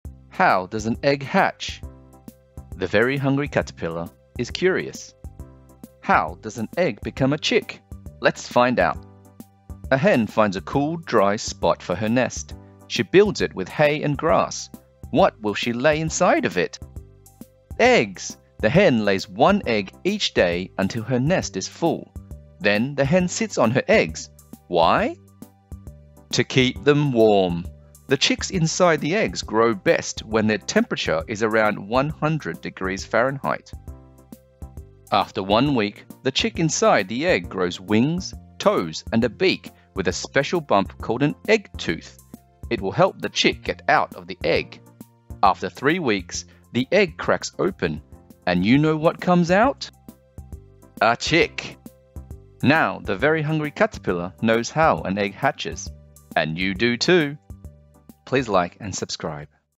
How Does An Egg Hatch? - Lifecycles with The Very Hungry Caterpillar - Kids Book Read Aloud
Learn how an Egg becomes a baby chick with The Very Hungry Caterpillar!

In this nonfiction story, young readers explore the transformation of an egg into a chick. The miracles of nature come to life in this early-learning series centred around life cycles, featuring simple text and Eric Carle's classic illustrations!

You can purchase this book and other The Very Hungry Caterpillar titles here on Amazon: